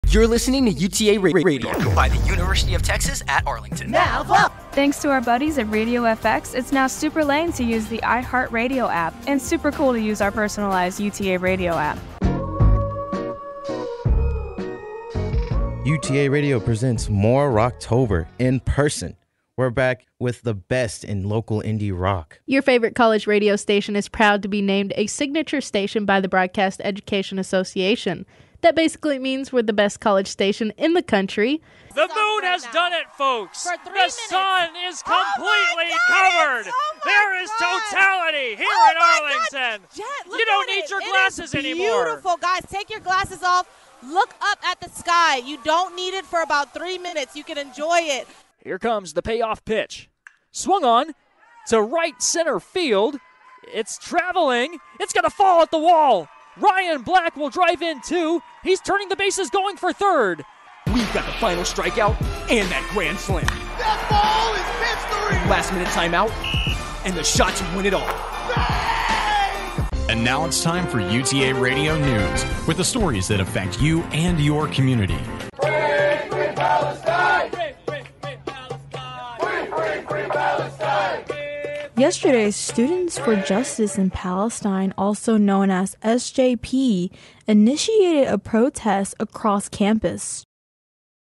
UTA Radio Snapshot 2023-24
A listen back at the this past year on UTA Radio